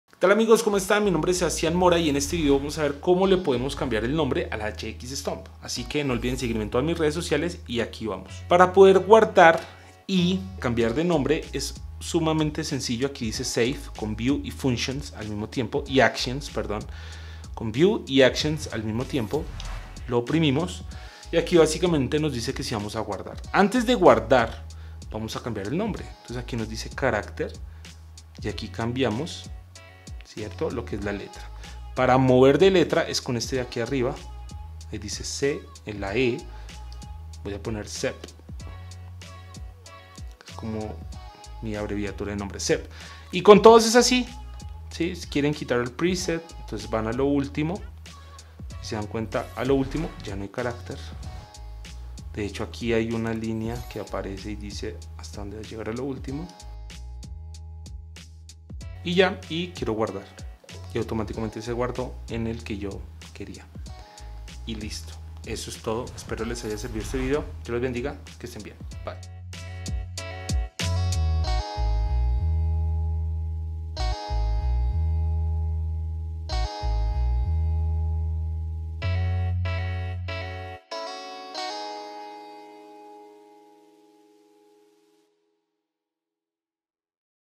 ¿Como GUARDAR y Cambiar de Nombre tus PRESETS? HX Stomp de Line 6 ► Sebastian Mora
First God

// FOLLOW ME \\